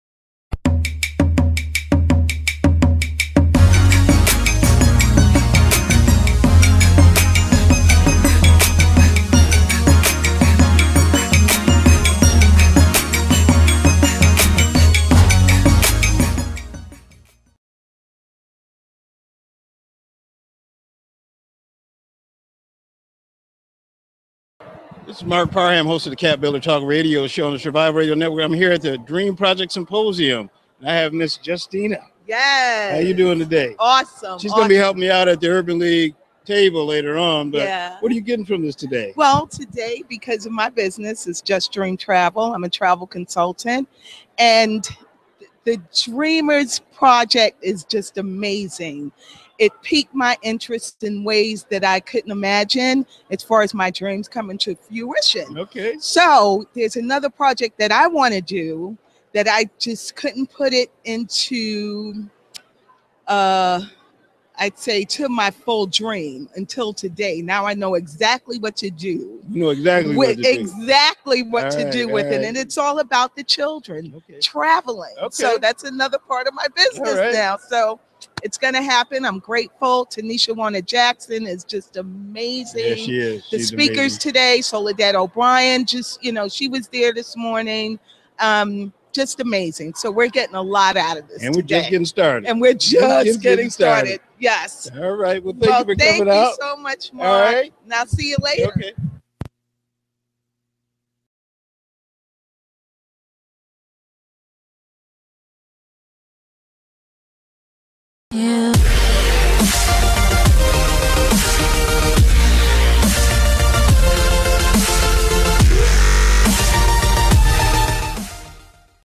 Justina Murphy-Pauls at the Dream Project symposium
Travel Agent Justina Murphy-Pauls at the Dream Project symposium w. SRN's Marc Parham.